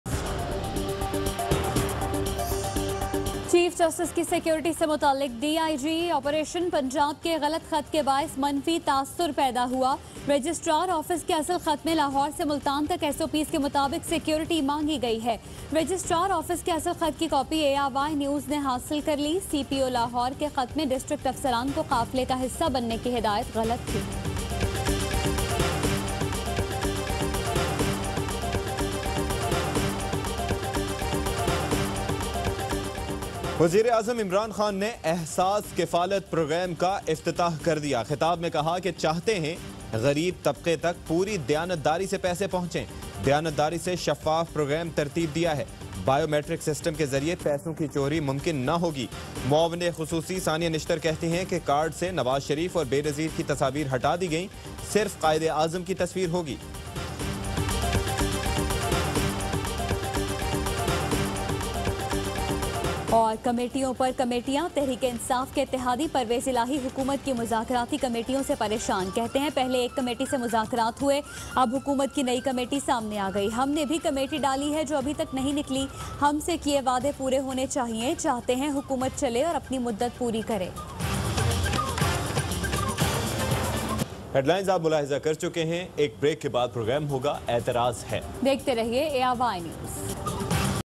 ARYNews Headlines |Zalmay Khalilzad arrives in Islamabad on a day-long visit| 8PM | 31 Jan 2020
Two IT parks being set up in Pakistan, govt informs NA.

NAB summons PML-N lawmaker Javed Latif on February 4.

PM aide explains why political figures’ pictures removed from Kafaalat card.

FIA launches inquiry into BISP scam.

PML-N MPA declared ineligible over possessing fake degree.

Zalmay Khalilzad arrives in Islamabad on a day-long visit.

Mainly dry and cold weather to persist in most parts of country.

PML-N leader challenges PM aide to prove kickback charges against Nawaz.

Women’s property rights, national accountability ordinances laid before NA.

FM Shah Mahmood concludes Kenya visit.